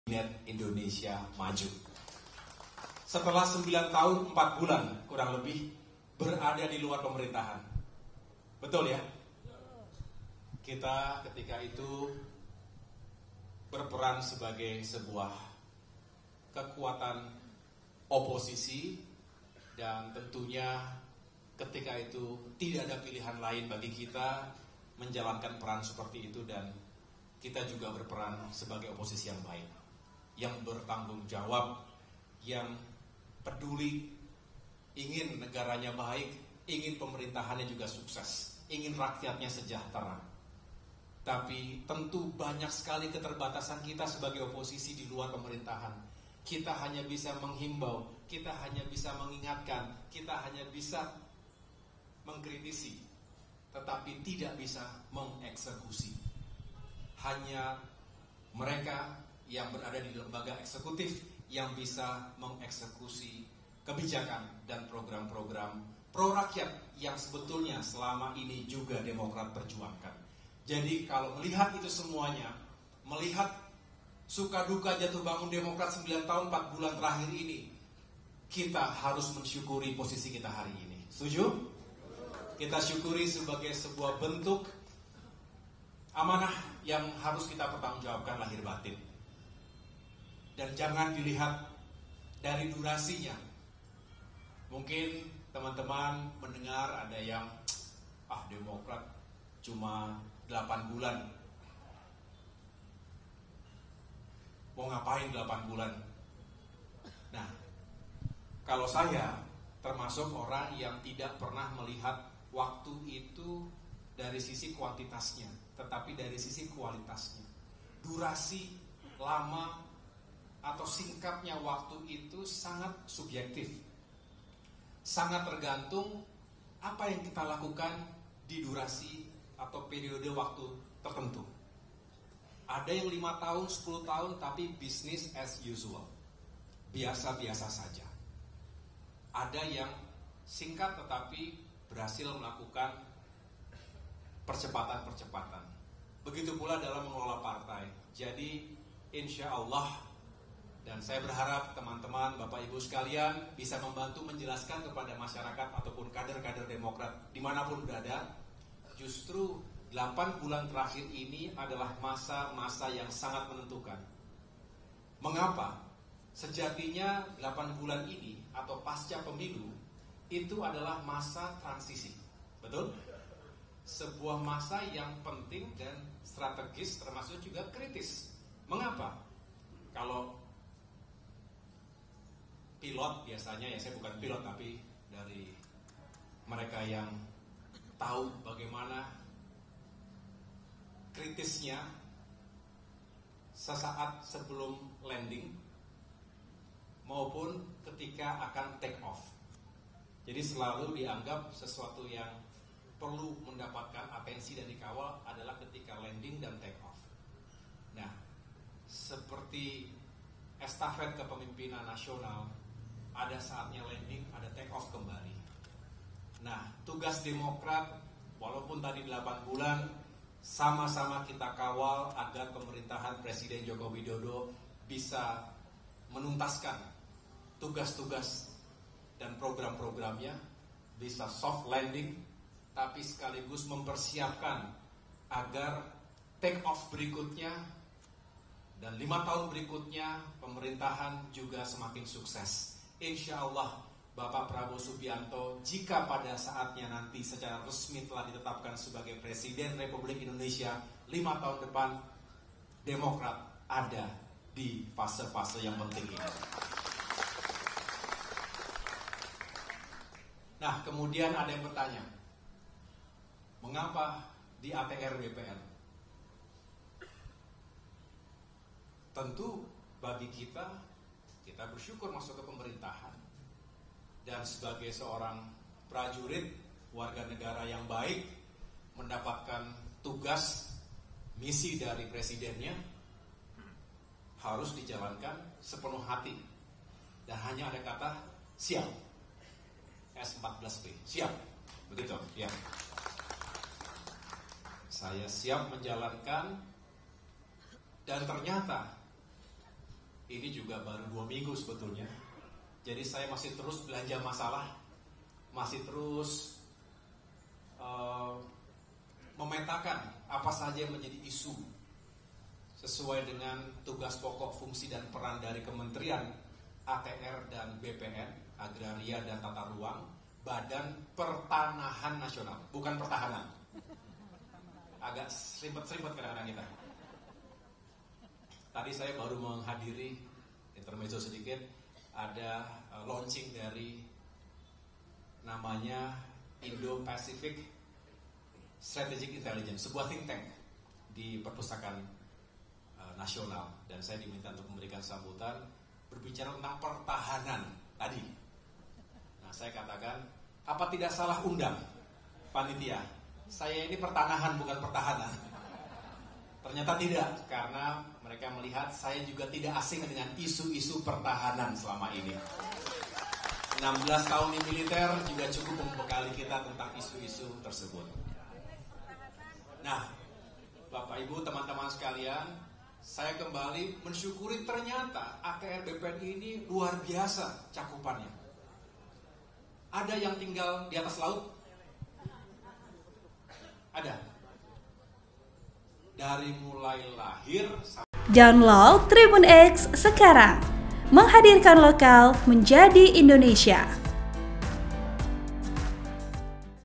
JAWABAN MENOHOK AHY Kala Disinggung Hanya Jadi Menteri Selama 8 Bulan : Mau Ngapain
TRIBUN-MEDAN.COM - Menteri ATR/BPN sekaligus Ketum Partai Demokrat Agus Harimurti Yudhoyono (AHY) memberi jawaban menohok usai disinggung waktunya bergabung dengan Kabinet Indonesia Maju hanya selama 8 bulan.

Hal itu disampaikan AHY saat dengan Pengurus DPP di Kantor DPP Partai Demokrat, Jumat (8/3/2024).